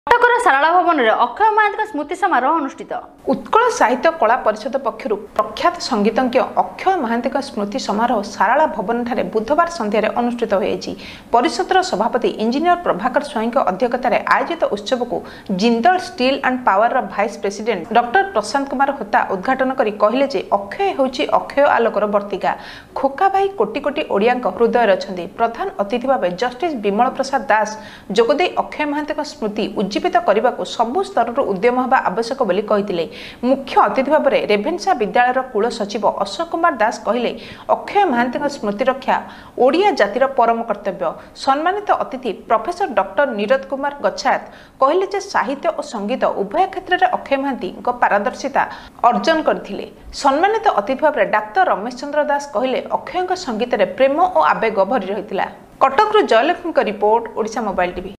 14th Dec 2018 II Akshay Mohanty Smruti Sabha News II Justice Bimal Prasad Das Chief Guest
Dr. Er. Pravakar Swain II Sarala Bhawan Cuttack